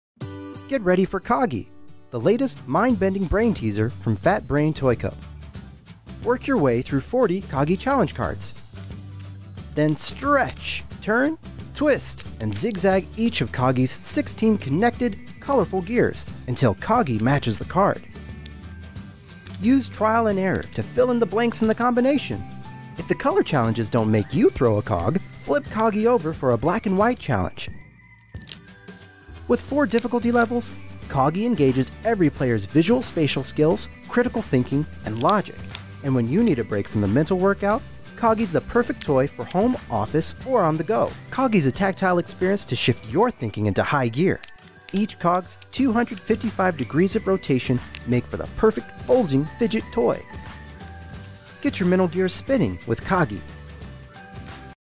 Fat Brain Toys Coggy
Get your mental gears spinning with this bending, clicking, shape shifting brainteaser. Moving from one challenge card to the next, it’s up to you to fold this connected collection of sixteen colorful gears into the perfect positions that match each image.